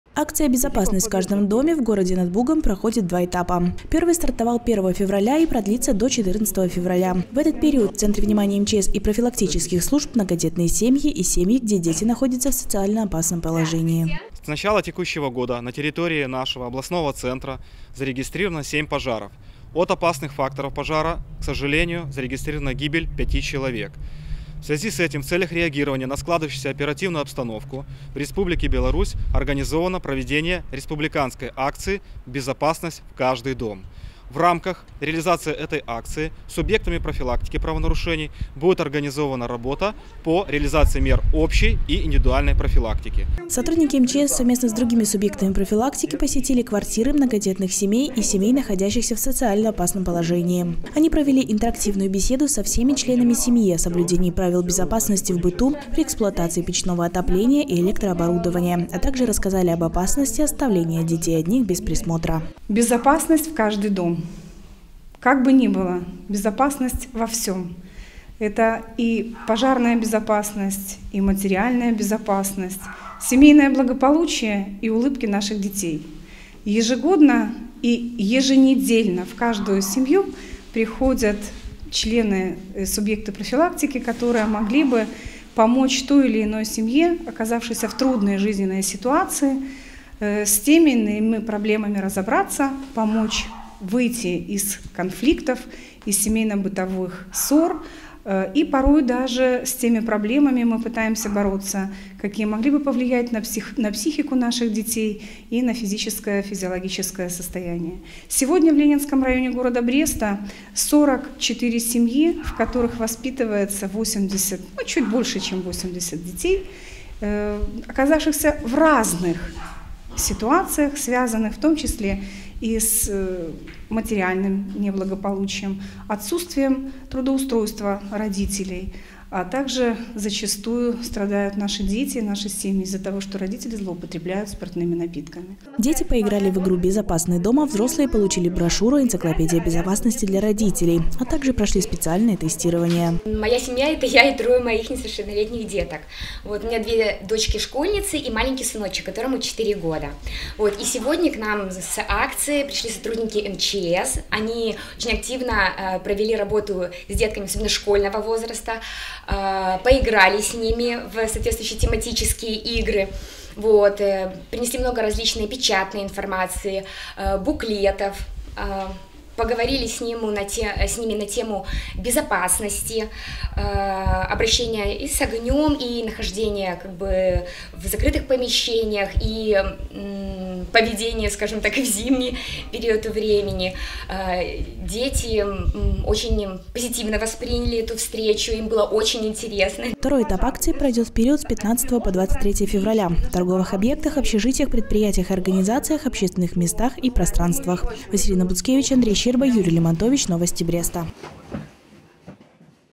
2024-02-02 Акция МЧС «Безопасность в каждый дом»
С целью предупреждения пожаров и гибели людей 1 февраля в Бресте был дан старт республиканской информационно-пропагандистской акции «Безопасность в каждый дом». В рамках профилактической работы специалисты МЧС посетят дома и квартиры, где проведут интерактивную беседу с многодетными семьями и семьями, находящимися в социально-опасном положении. Пожарные рассказывают о правилах пожарной безопасности и дают рекомендации по действиям в случае возникновения чрезвычайной ситуации. В первый рейд, в рамках акции отправилась и наша съёмочная группа.
*Наши контакты:*
✅Телефон горячей линии:
54-63-67